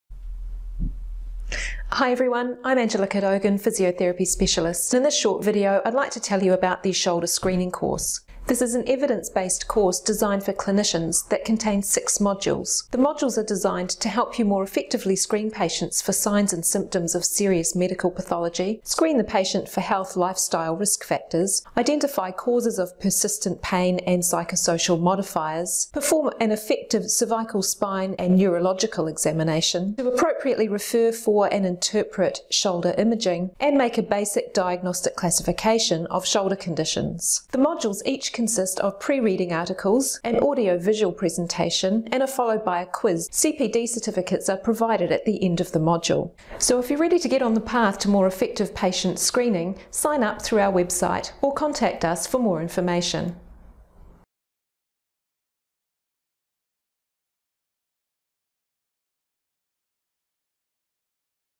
Shoulder Screening Course_2019
This is an online course for physiotherapists covering the screening of patients presenting with upper limb pain. The course covers red flags, health screening, pain and psychosocial modifiers, cervical spine examination, shoulder imaging and diagnostic classification of shoulder conditions.